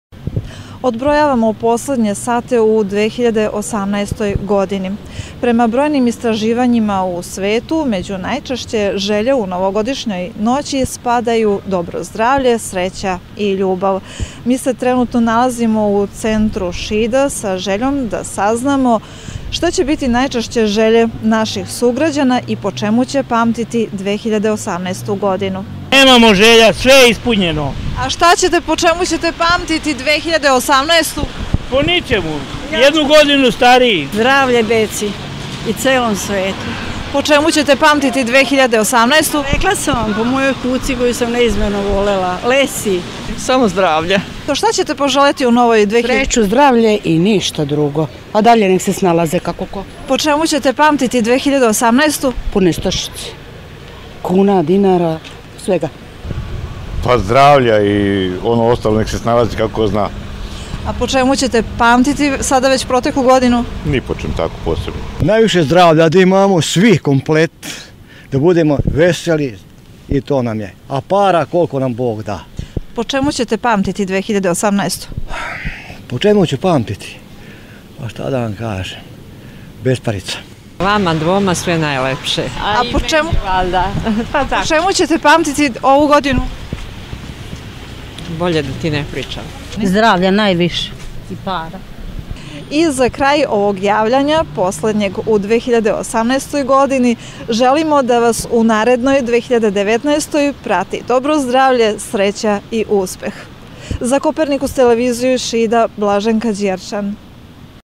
Novogodišnja želja anketa
Poslednji decembarski dan rezervisan je za novogodišnje želje i čestitke. U narednoj anketi saznajemo koje su najčešće želje stanovnika Šida za narednu godinu kao i to, po čemu će pamtiti prethodnu 2018